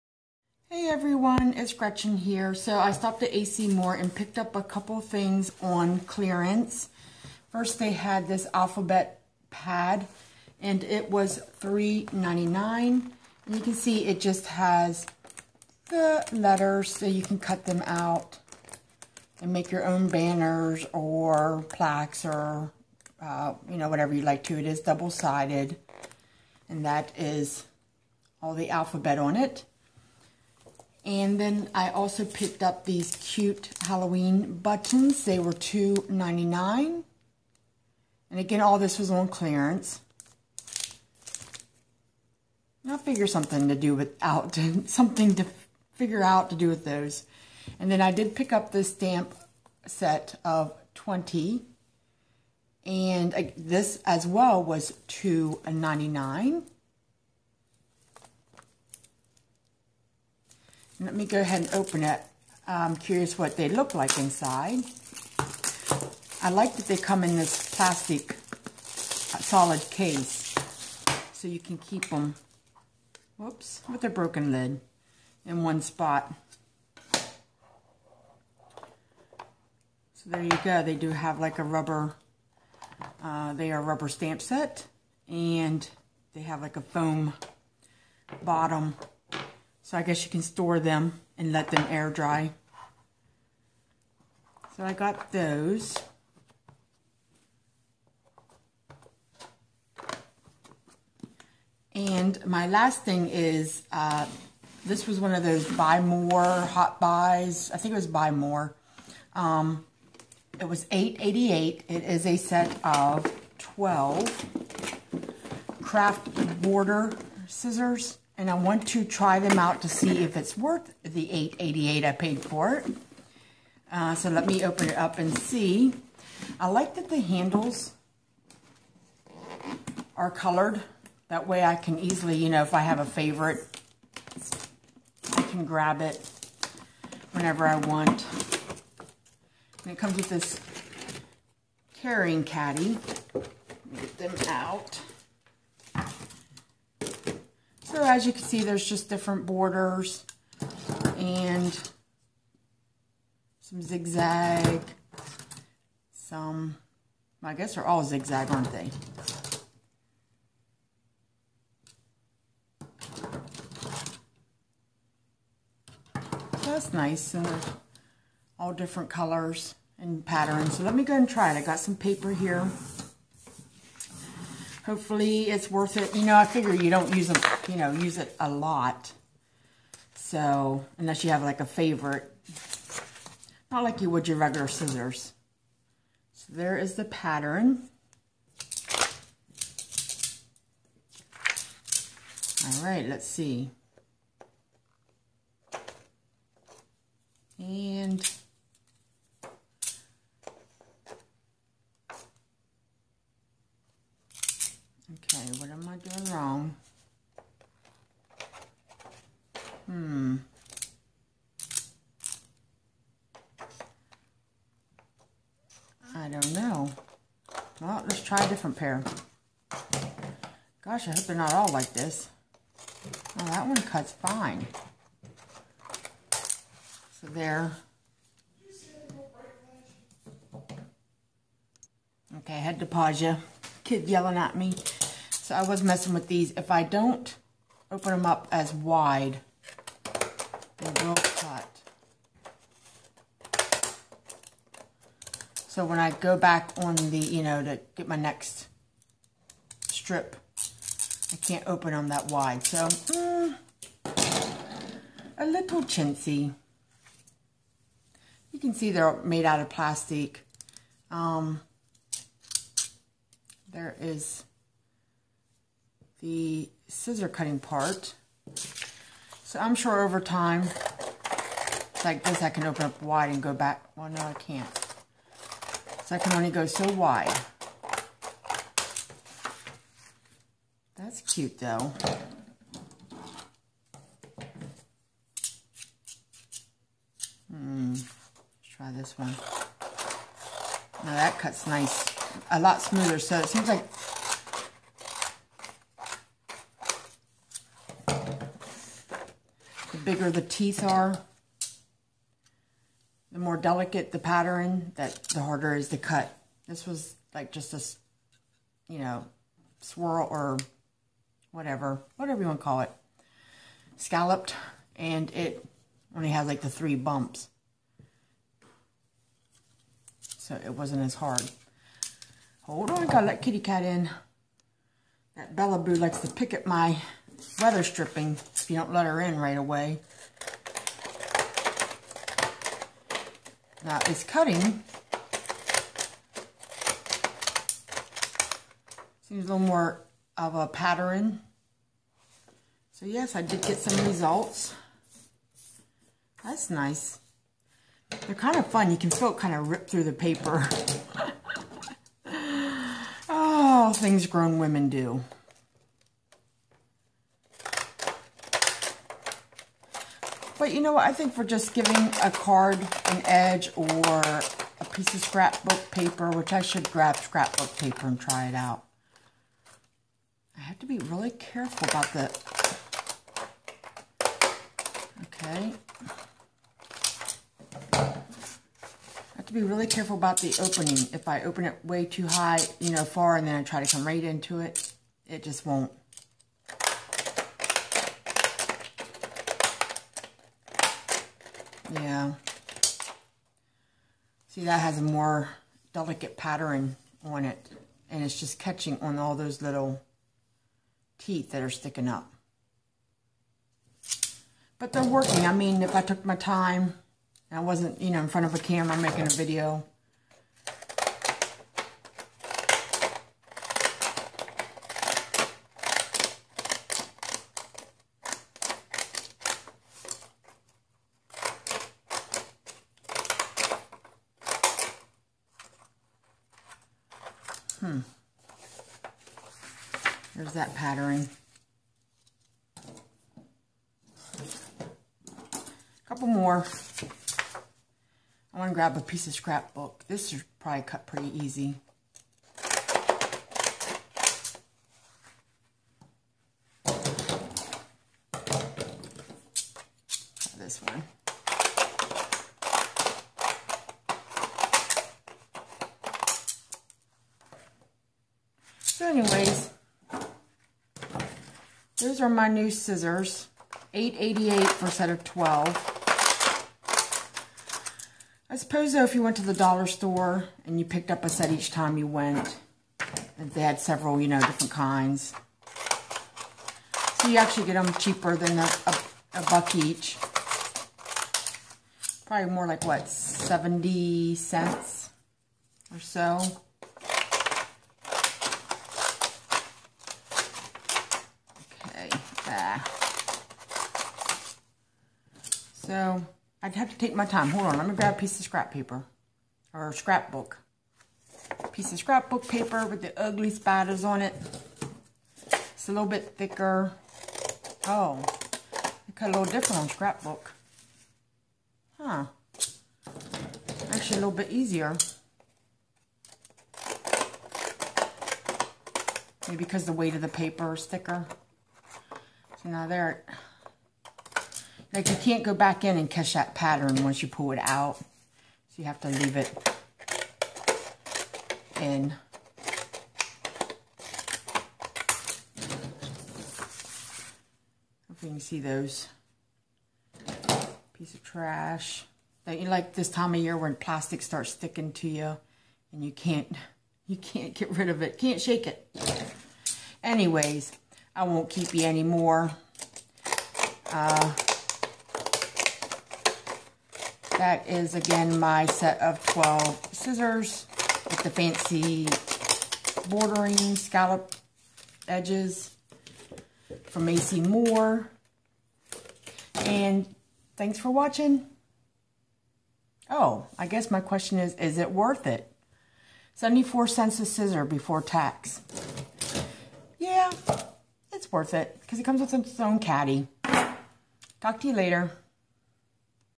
AC Moore Haul | Is It Worth It?
Small haul and trying out my new scissor caddy.

Snail-mail:
Gretchen – Catnpaws